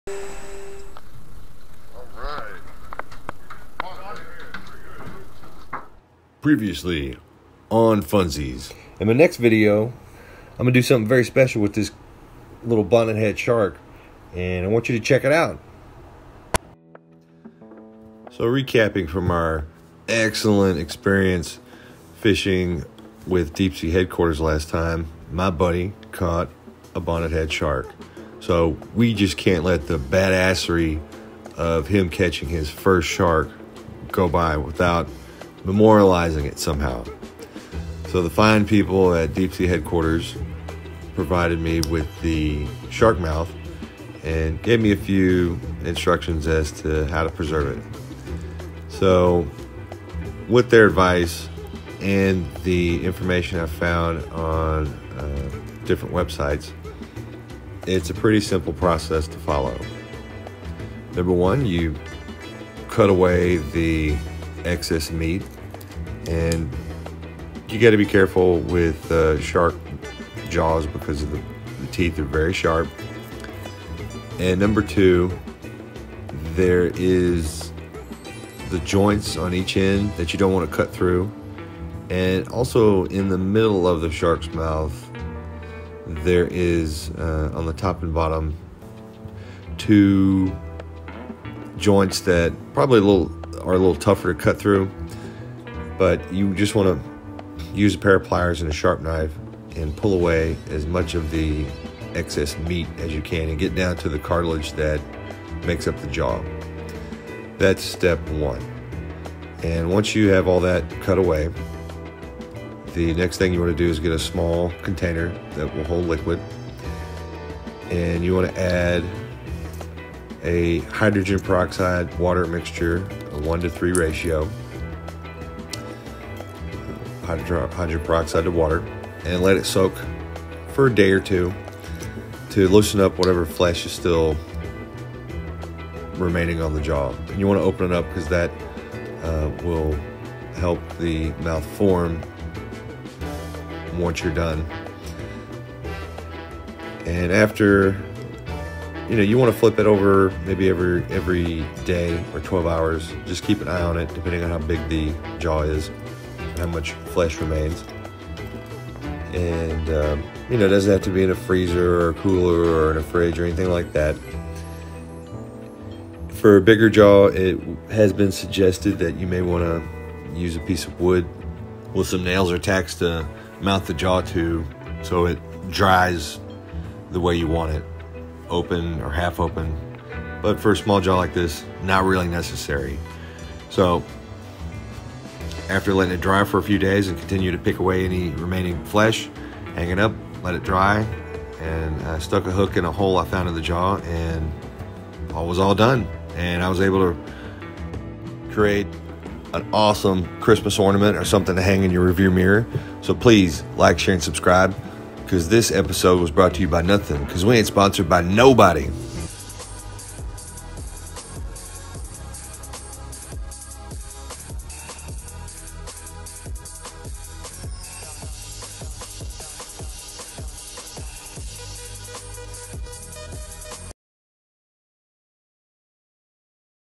How to Preserve a Shark Jaw 🎣🌊
Preserving a shark jaw (Bonnetthead Shark) is easy and fun! It costs almost nothing to do!